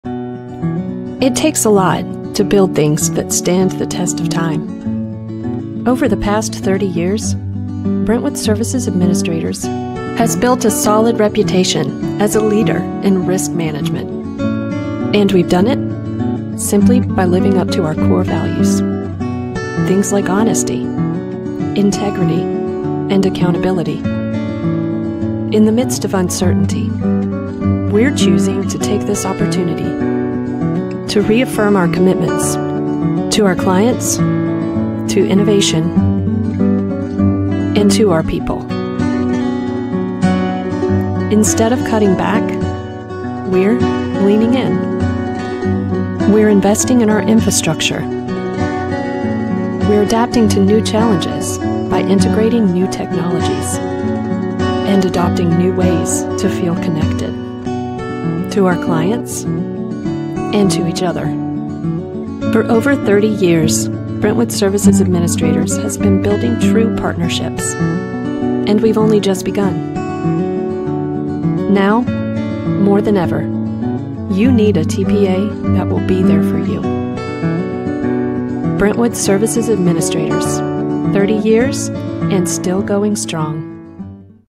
Branded COVID Video for Client (Brentwood) by 90 Degree Design
Our clients, Brentwood Services Administrators, asked us to help them create a branded video to share how they planned to handle COVID-19. They wanted something that showcased how they've continued to invest in their partnerships, clients, technology, and employees.